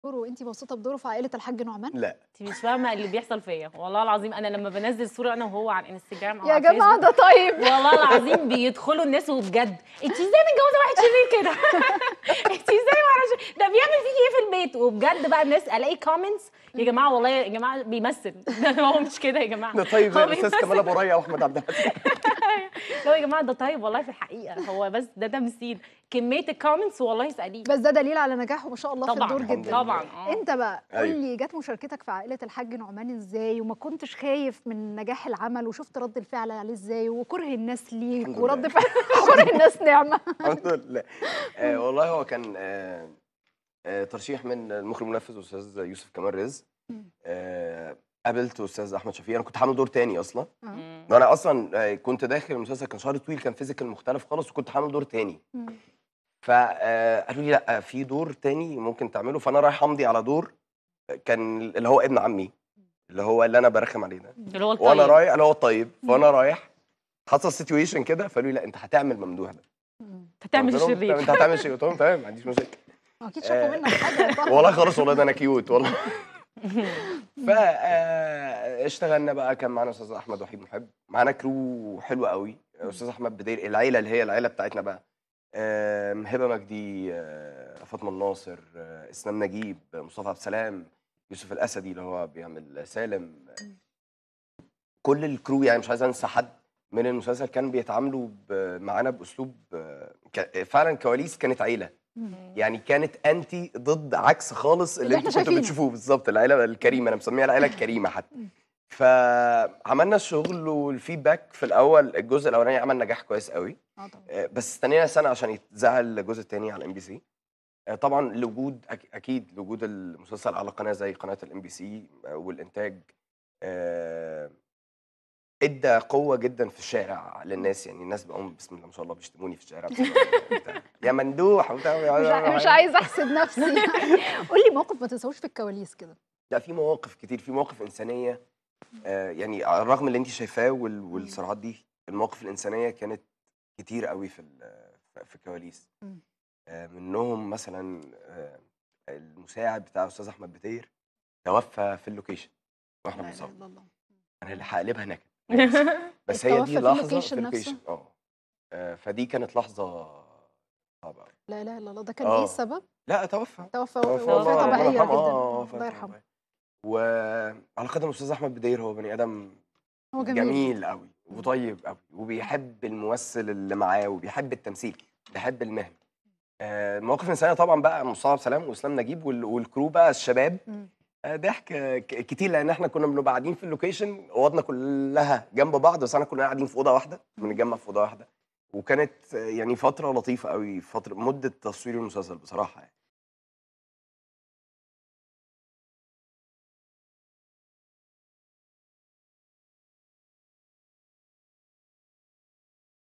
الفنان طه خليفة يكشف تفاصيل مشاركته في مسلسل "عائلة الحاج نعمان" ورأي زوجته ريم أحمد في دوره
ويمثل عودة لها لشاشة MBC وذلك بعد حصولها على درجة الدكتوراه.